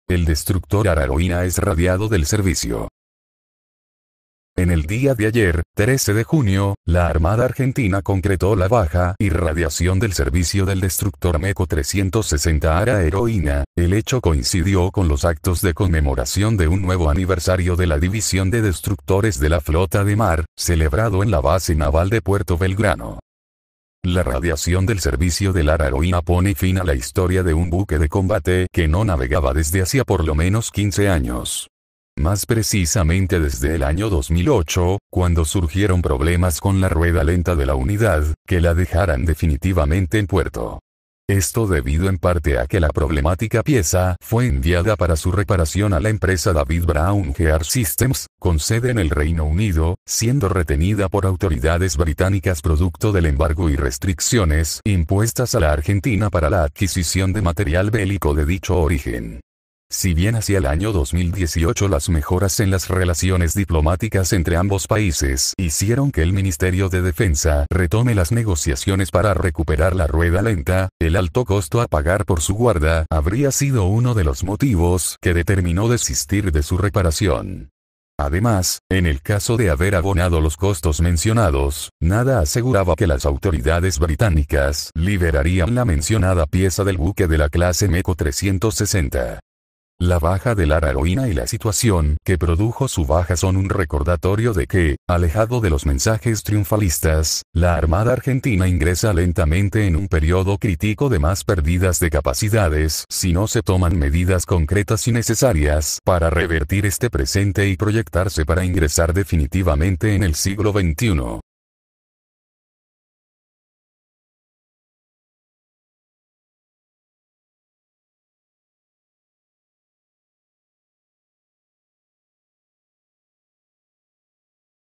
🇦🇷⚓El Destructor "ARA Heroina" (D-12) Fue Radiado del Servicio
En el dia de Ayer, 13 de Junio, la Armada Argentina concretó la baja y radiación del servicio del destructor MEKO 360 ARA “Heroína”. El hecho coincidió con los actos de conmemoración de un nuevo aniversario de la División de Destructores de la Flota de Mar, en la Base Naval de Puerto Belgrano.

La radiación del servicio del ARA Heroína pone fin a la historia de un buque de combate que no navegaba desde hacia por lo menos 15 años. Más precisamente desde el año 2008, cuando problemas con la “rueda lenta” de la unidad la dejaran definitivamente en puerto. Esto debido en parte a que la problemática pieza fue enviada para su reparación a la empresa David Brown Gear Systems Ltd. con sede en el Reino Unido, siendo retenida por autoridades británicas producto del embargo y restricciones impuestas a la Argentina para la adquisición de material bélico de dicho origen.
Si bien hacia el año 2018 las mejoras en las relaciones diplomáticas entre ambos países hicieron que el Ministerio de Defensa retome las negociaciones para recuperar la rueda lenta, el alto costo a pagar por su guarda habría sido uno de los motivos que determinó desistir de su reparación. Además, en el caso de haber abonado los costos mencionados, nada aseguraba que las autoridades británicas liberarían la mencionada pieza del buque de la clase MEKO 360.

La baja del ARA Heroína y la situación que produjo su baja son un recordatorio de que, alejado de los mensajes triunfalistas, la Armada Argentina ingresa lentamente en un periodo critico de más perdidas de capacidades si no se toman medidas concretas y necesarias para revertir este presente y proyectarse para ingresar definitivamente en el siglo XXI.